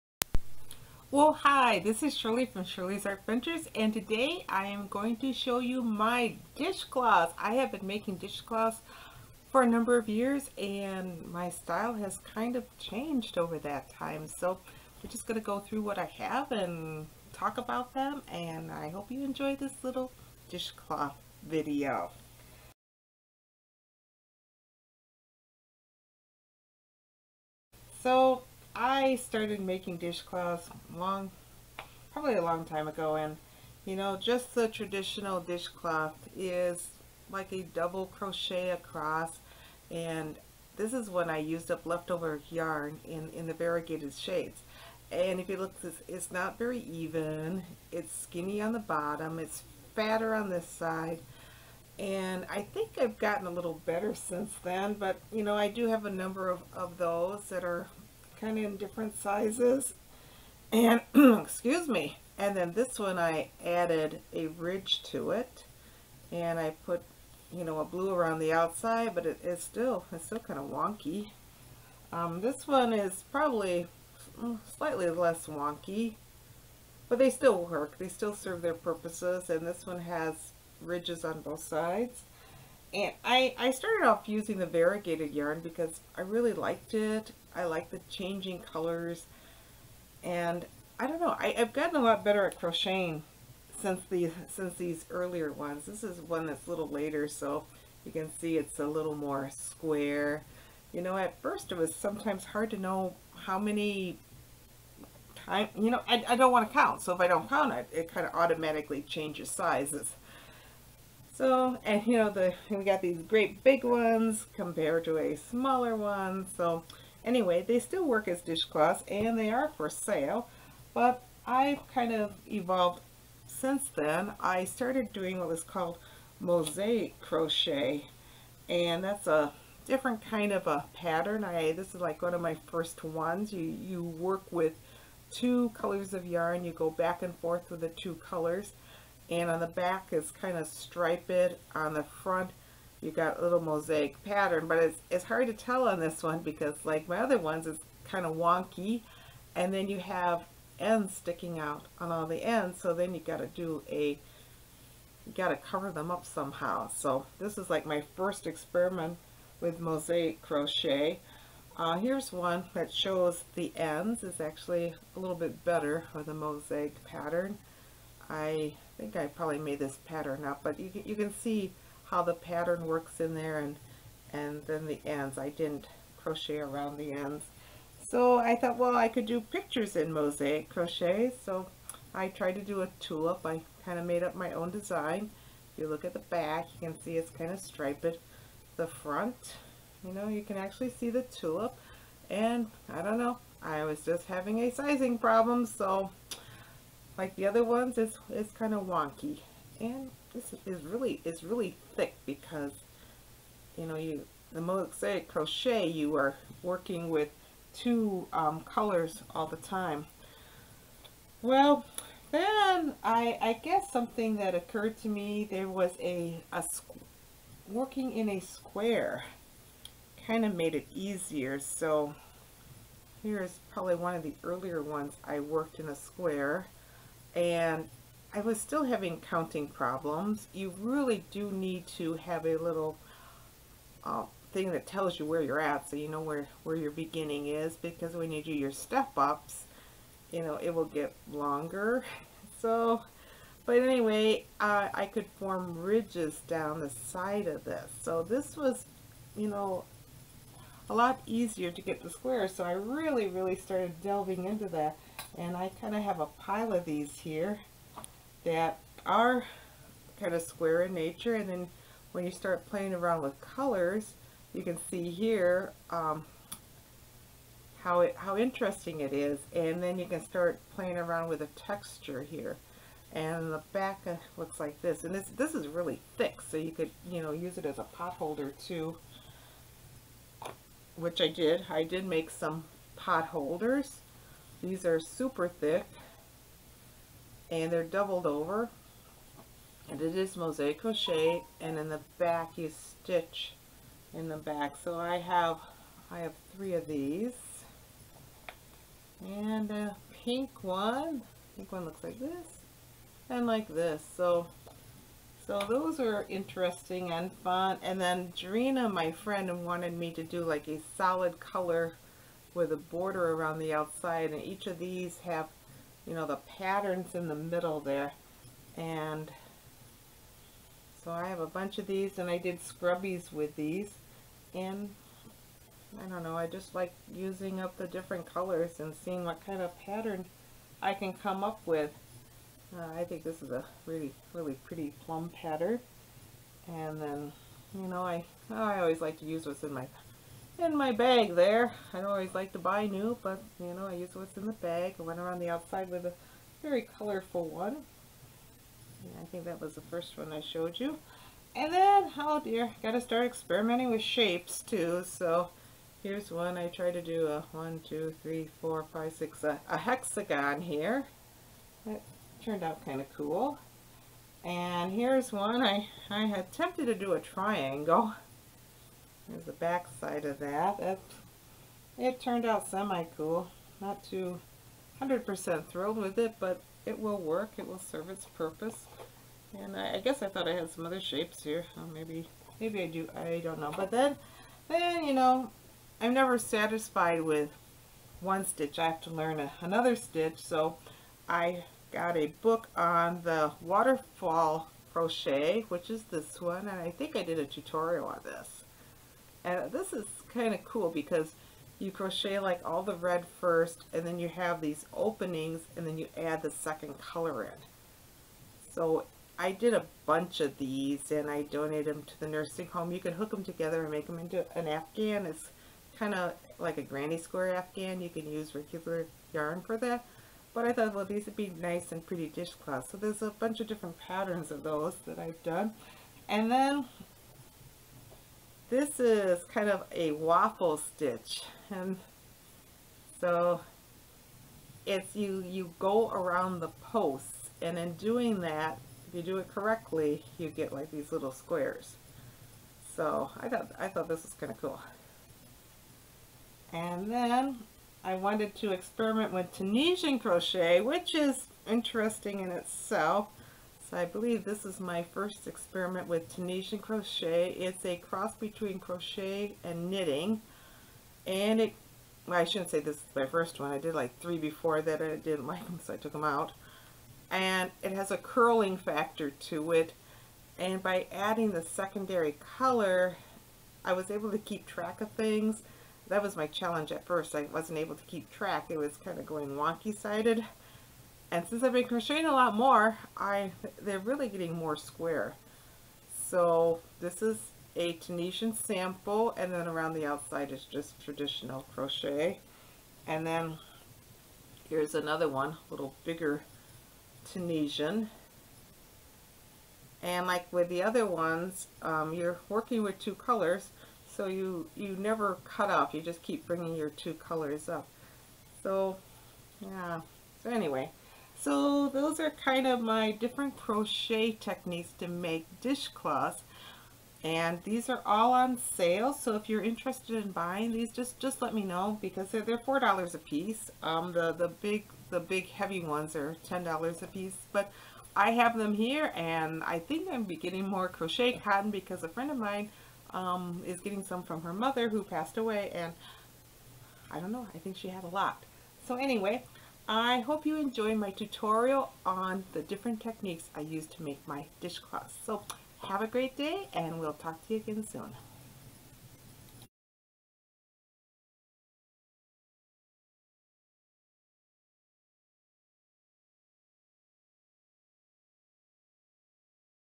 Crochet Styles with Dishcloths
I have been learning different crochet techniques while making my dishcloths.  This video showcases what I have in my stash.  Check out how my technique has changed.